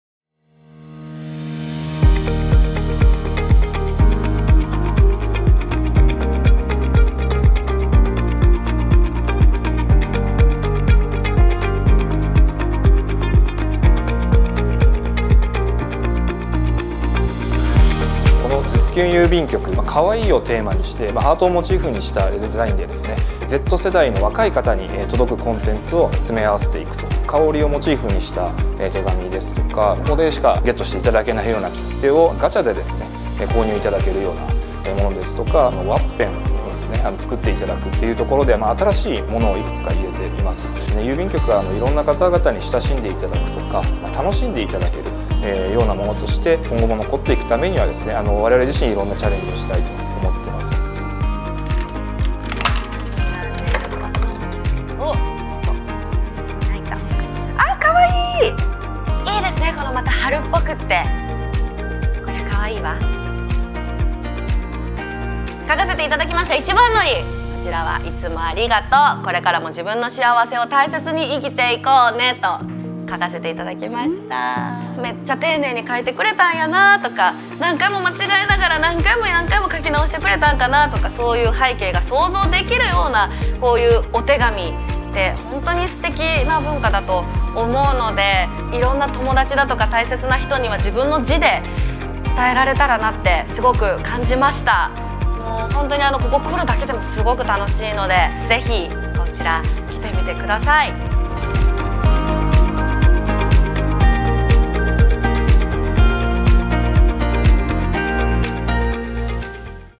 郵便局をもっと楽しく、ポップアップストア「ズッキュン♡郵便局」が大阪ルクアに登場
日本郵政と日本郵便は3月21日、郵便局に馴染みのない若い世代への新たなアプローチを目的としたポップアップストア「ズッキュン♡郵便局 in Osaka」のメディア向けオープニングイベントをルクア大阪にて開催した。

アンバサダーに就任した山之内すずさんは「SNSやテキストで色々送れる時代にはなっていますが、あらためて自分の想いを字に反映出来ると思います。めっちゃ丁寧に書いてくれたんやな〜とか、間違いながら何回も何回も書き直してくれたのかな〜とか、そういう背景が想像できるようなお手紙って本当に素敵な文化だと思うので、大切な人に送ってください」と手紙の大切さを語った。